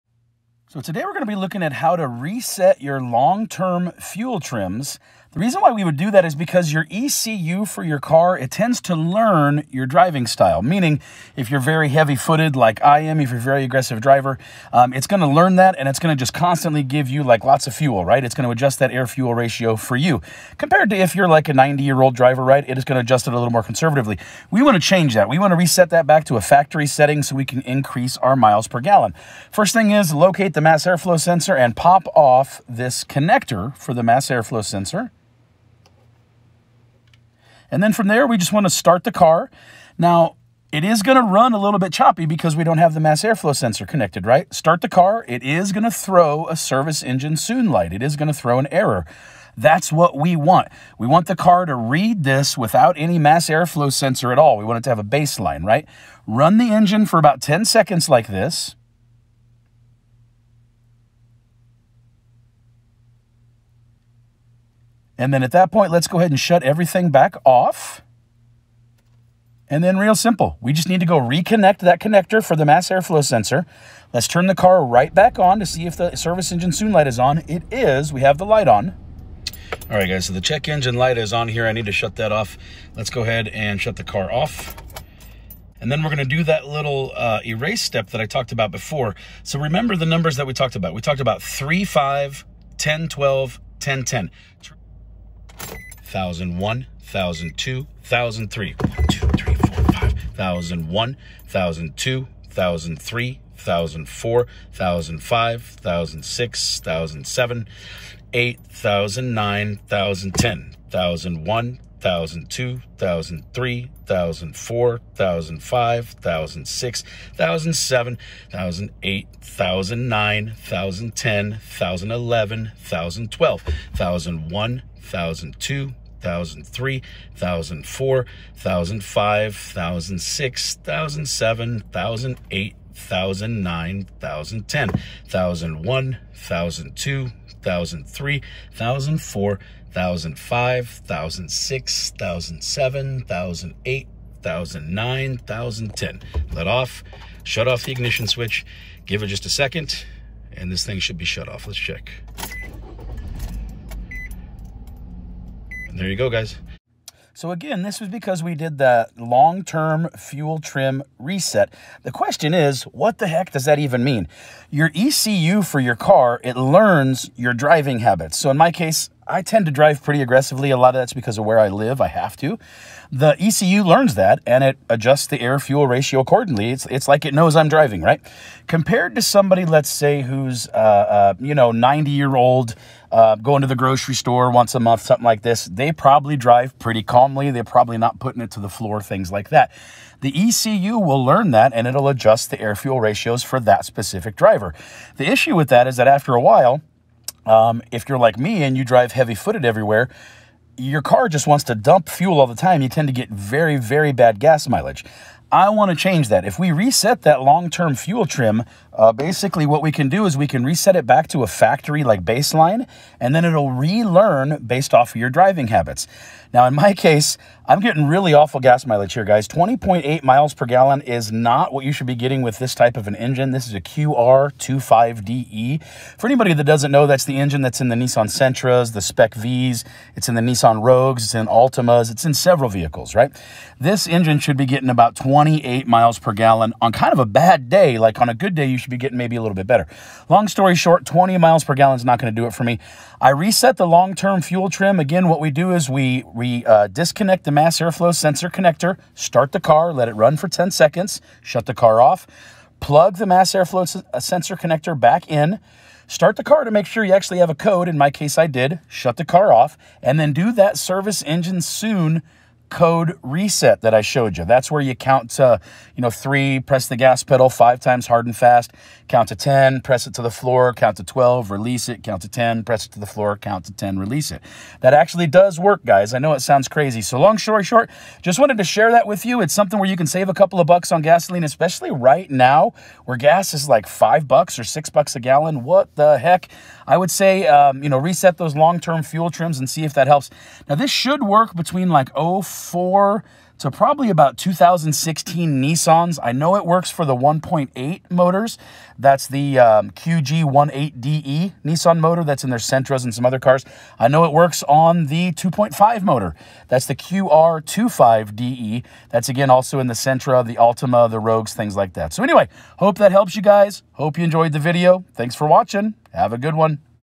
STOP WASTING GAS! How to reset your long term fuel trims.
Today I show you how to reset your long term fuel trims, which allows the ECU to relearn the fuel trims.